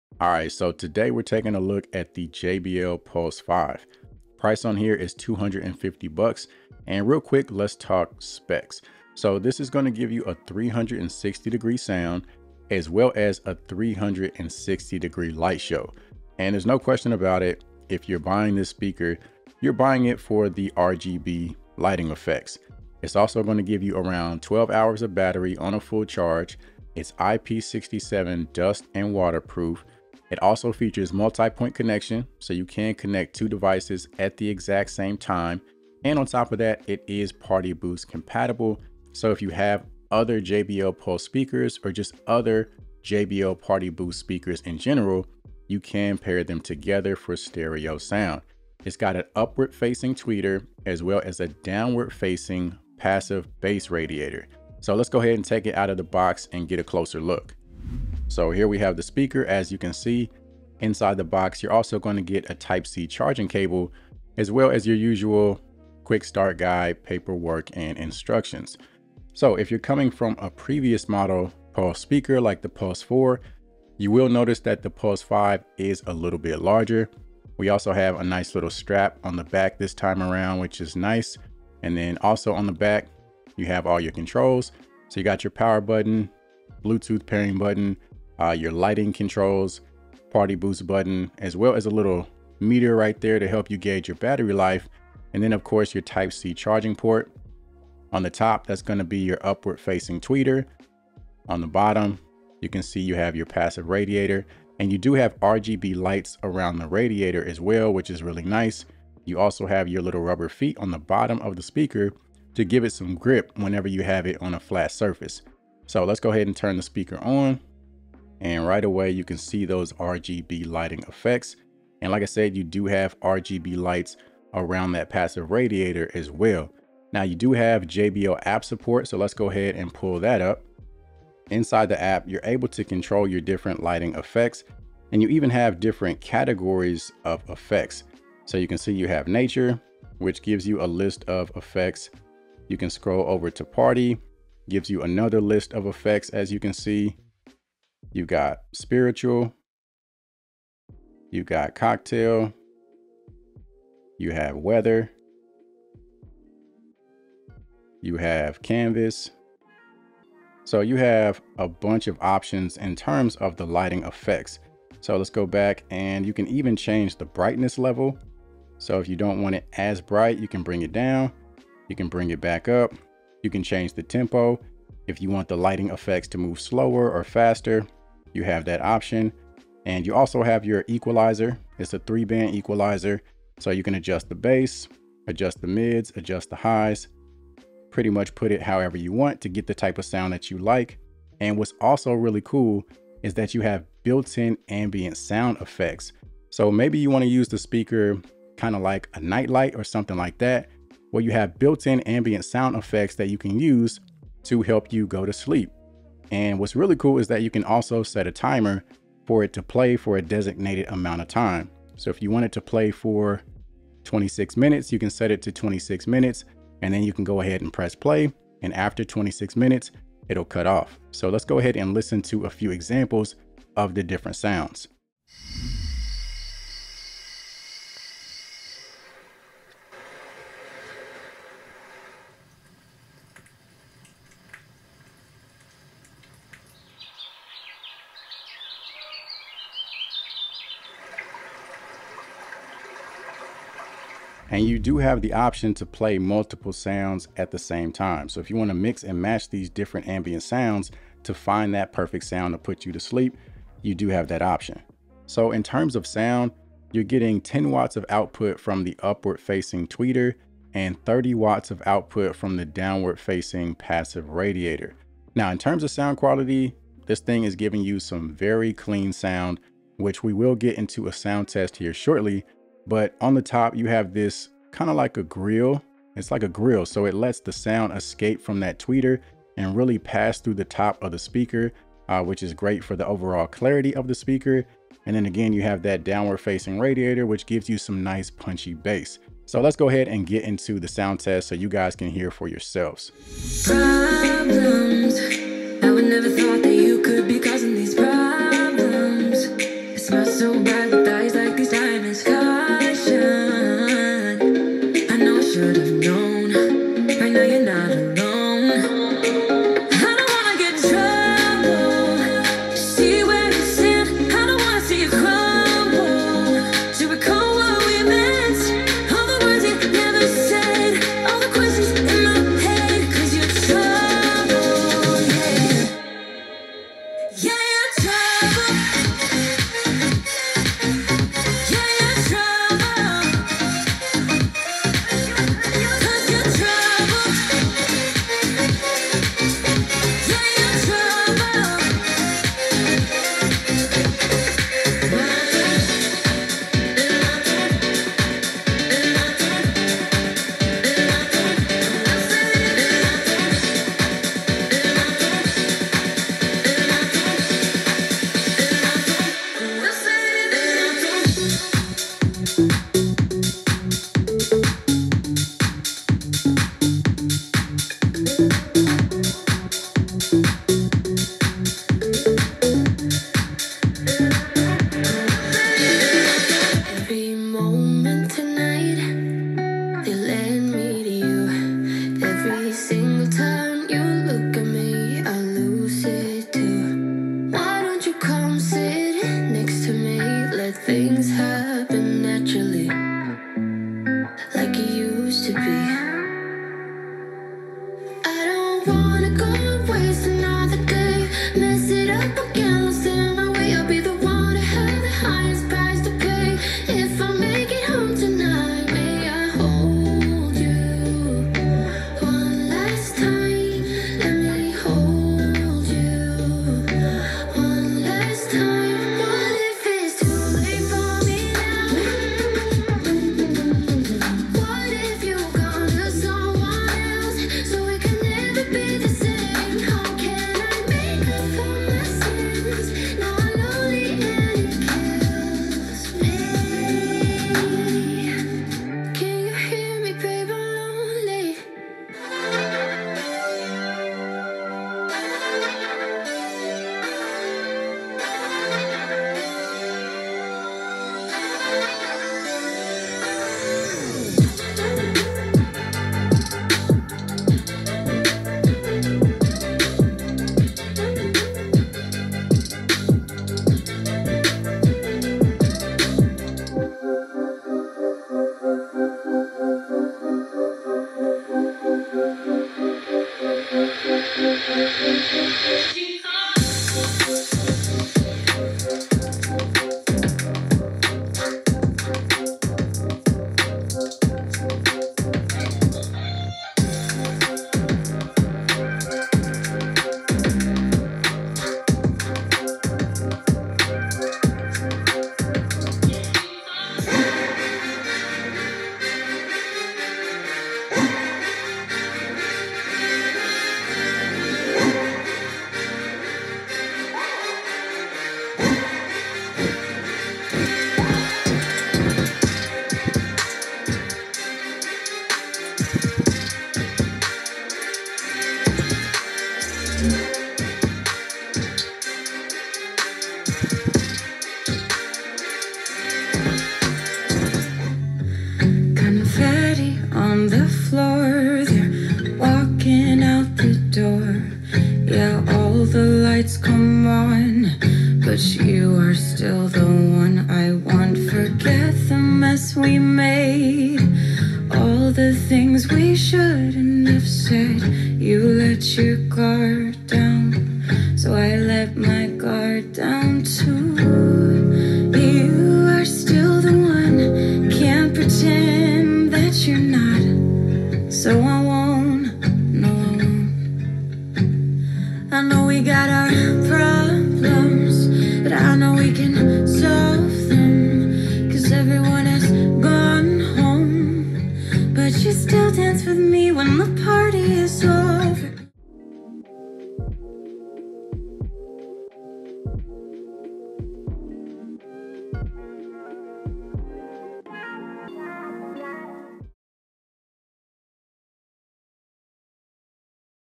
JBL Pulse 5: The Coolest Bluetooth Speaker You'll EVER Use!
CONNECT

What's going on YouTube! Today We're taking a look at the new JBL Pulse 5 Bluetooth Speaker. Also, let me know what products you'd like to see on the channel!

PURCHASE LINKS
• JBL Pulse 5

TIMESTAMPS
0:00 Intro
0:09 General Specs
0:58 Design & Build
2:11 JBL App
4:53 Sound Test 1
5:44 Sound Test 2
6:16 Sound Test 3
7:22 Sound Test 4
7:46 Sound Test 5
8:16 Sound Test 6
8:46 Sound Test 7